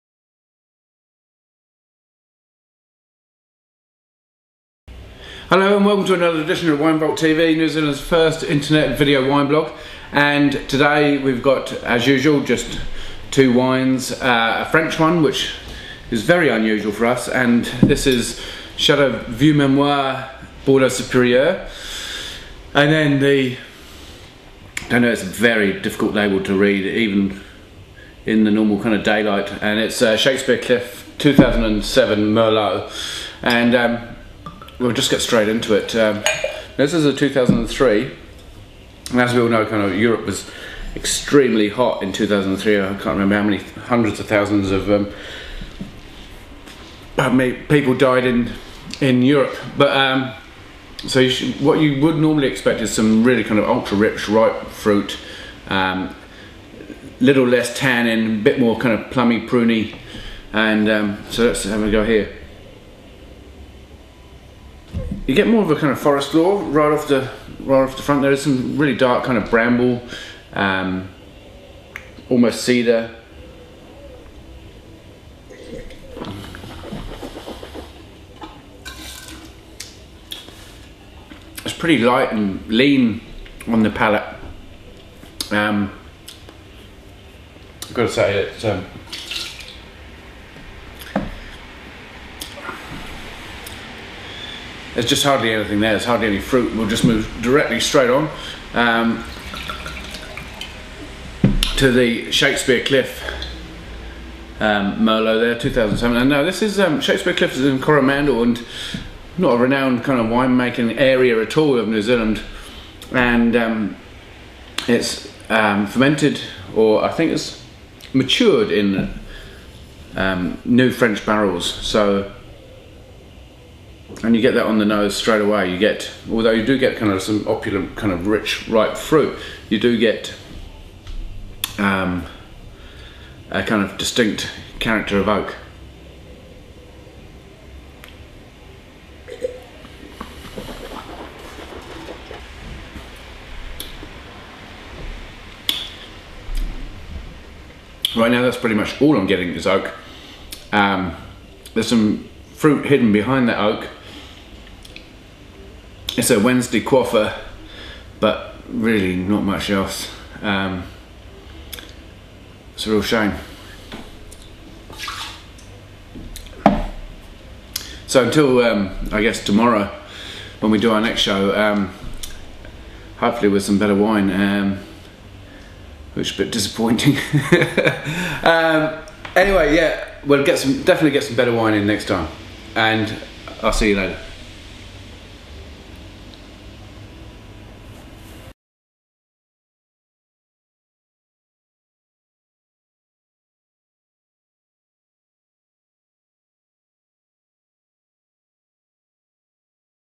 Big Bad Wednesday - Wine Vault TV Episode # 47
Jayson Bryant, of The Wine Vault, tastes 2 wines bad enough for me not to mention their names here!